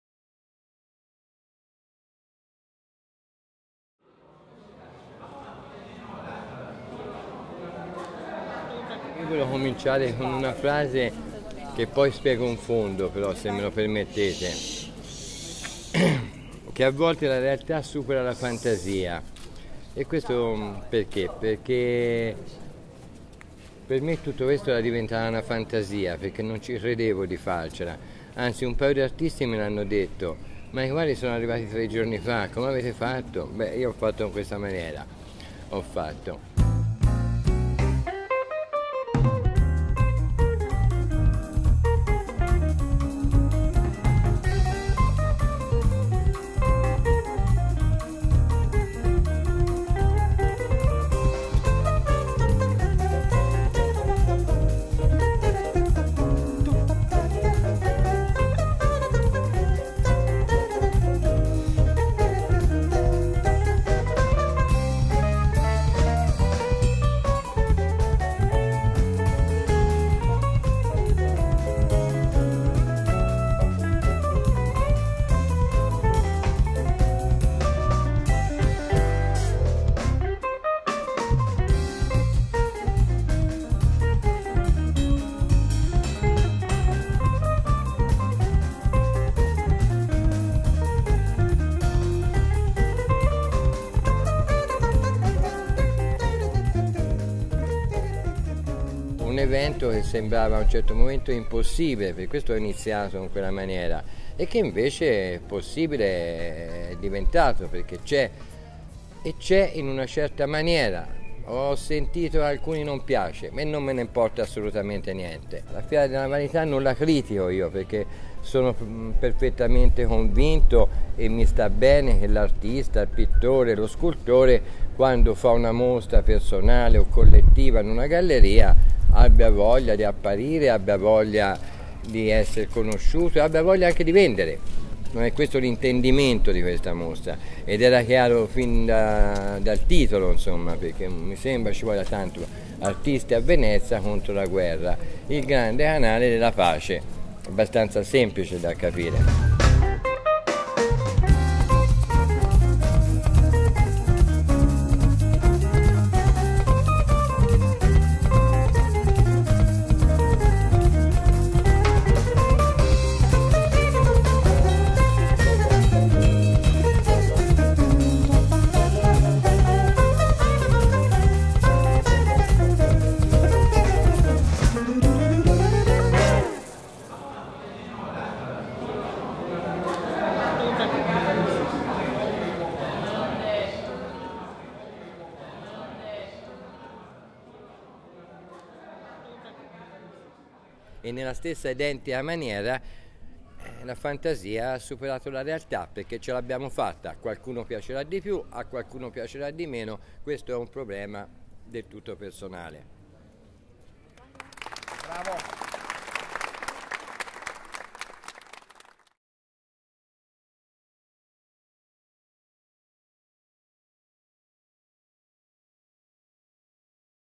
Il Grande Canale della Pace - 1a edizione
Il Grande Canale della Pace® è un progetto internazionale che ha l’obiettivo di dare voce all’arte di tutto il mondo contro la guerra.
L’iniziativa nasce a Venezia, il Canal Grande dell’arte universale, e da lì si propagherà in Europa e nel Mondo.
Questa prima edizione coinvolgerà artisti di tutti i Paesi, vedrà l’illustre e attiva partecipazione del premio Nobel per la Letteratura Dario Fo e dell’artista Romina Power e si avvarrà del patrocinio della Fondazione di Óscar Arias Sánchez, premio Nobel per la Pace.
Il Grande Canale della Pace®,
1a Edizione - Venezia, Palazzo Bollani, 9 maggio-22 novembre 2015.